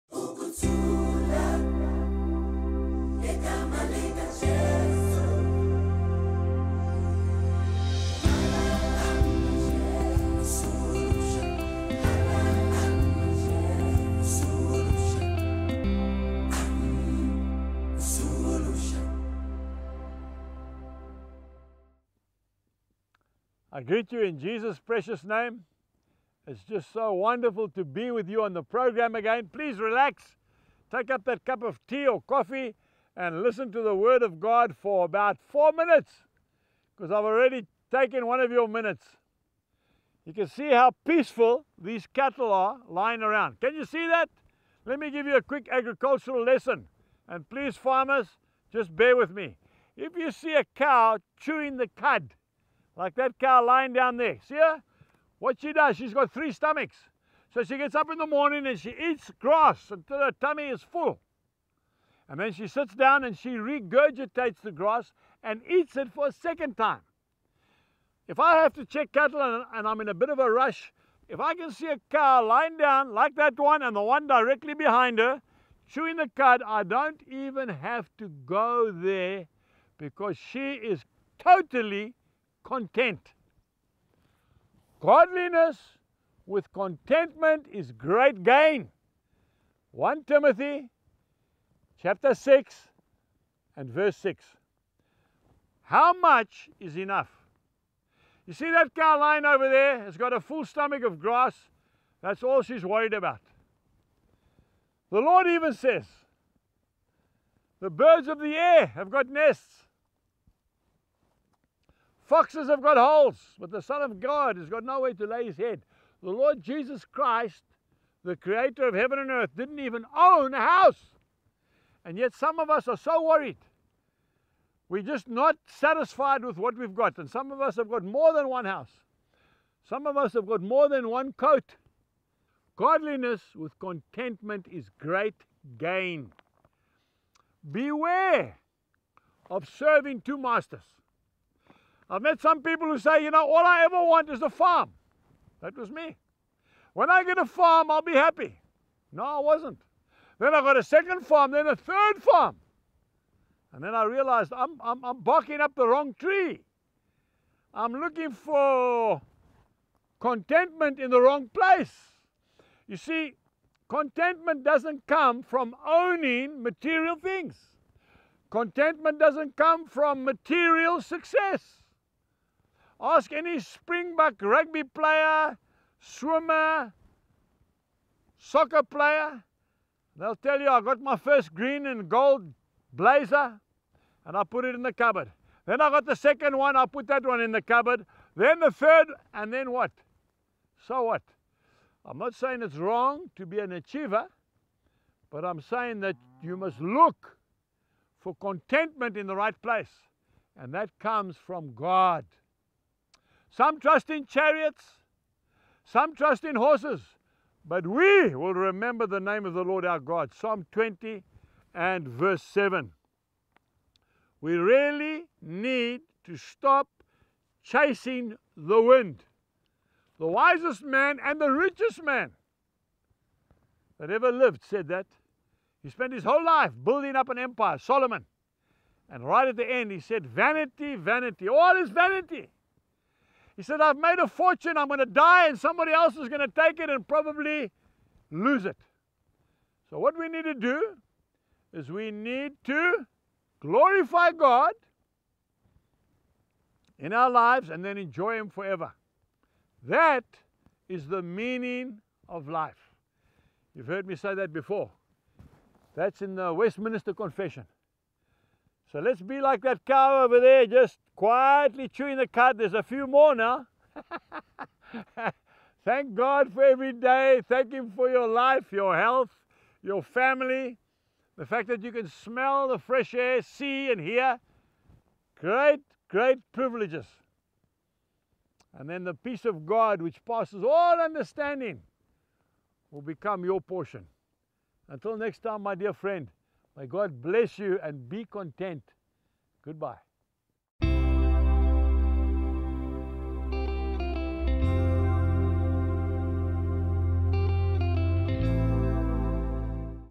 How Much is Enough - Family Time with Angus Buchan
Angus says that 1 Timothy 6:6 tells us that Godliness with contentment is great gain. In other words, if we’re chasing after money or looking for contentment in material things, then we’re wasting our time. Turn your energy and effort towards pleasing God and you will discover that true contentment.